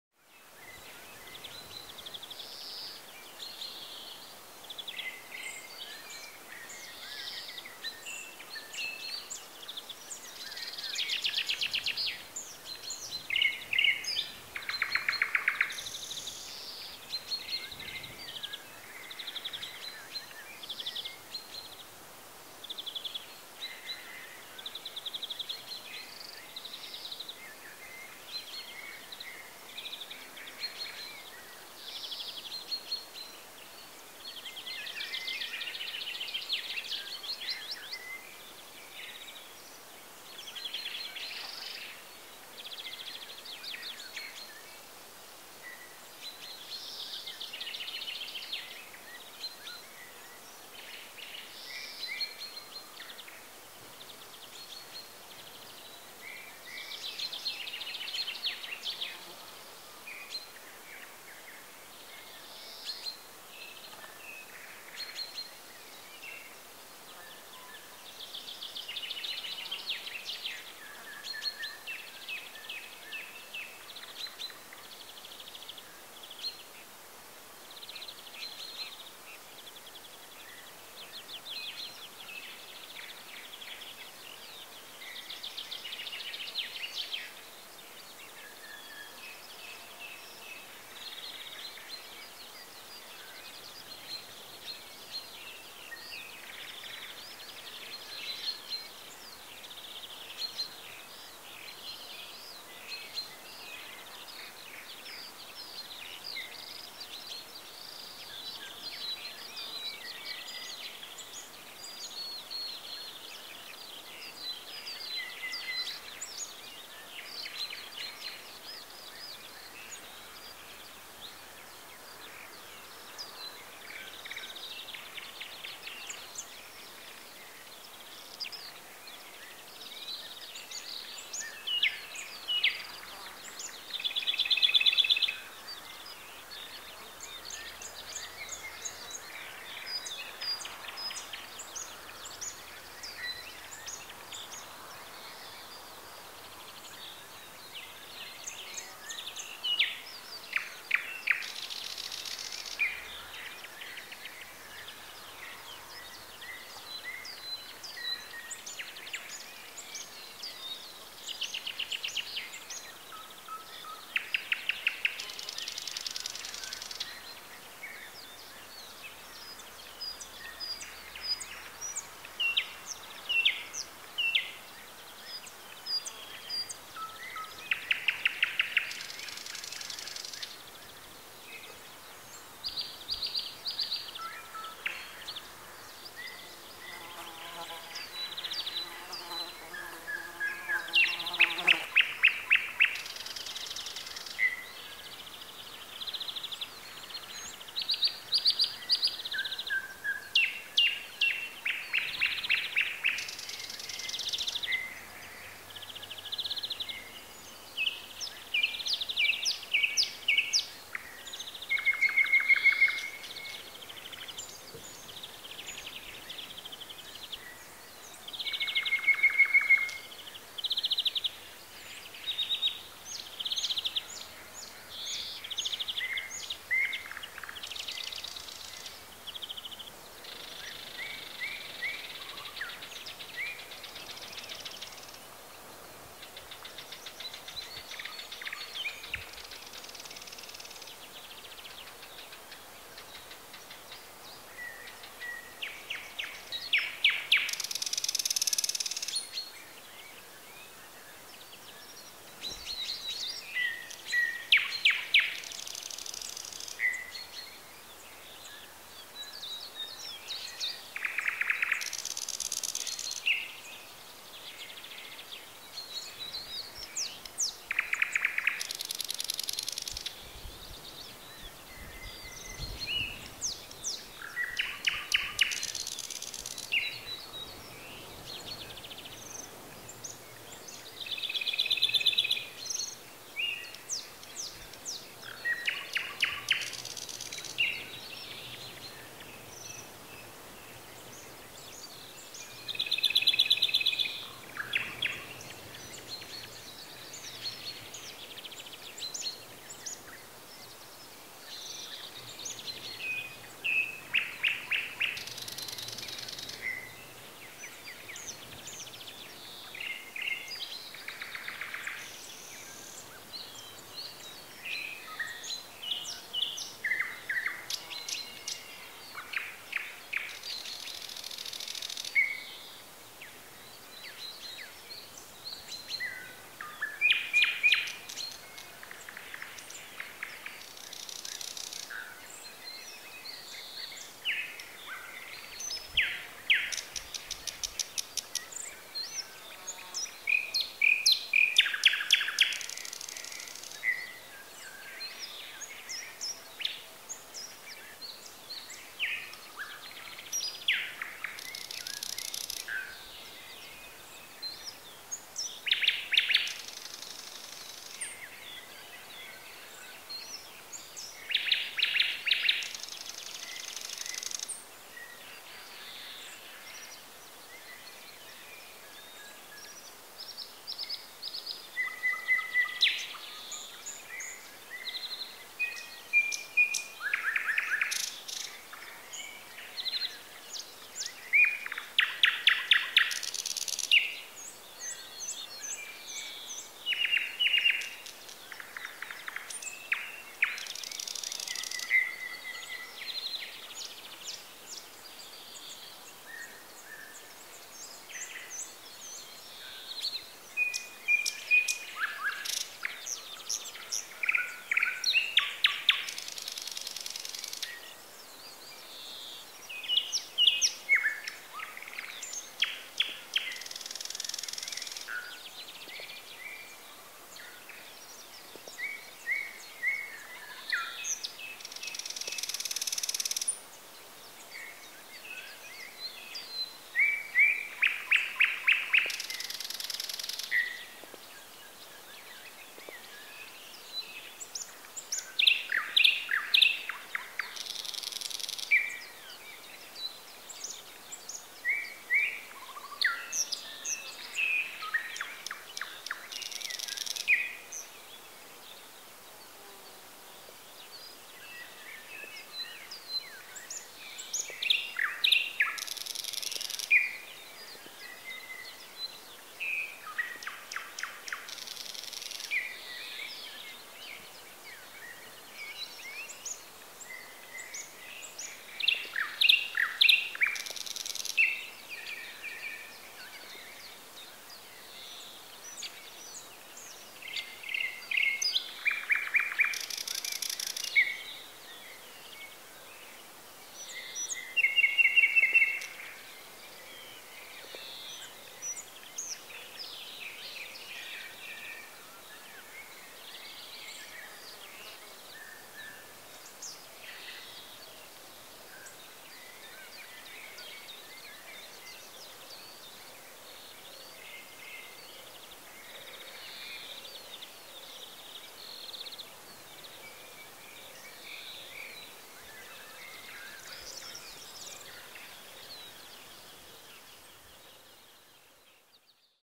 relax Sounds of nature. Birds. Звуки природы.Птицы
Ukraine, Bessarabia.
sound using Zoom H4n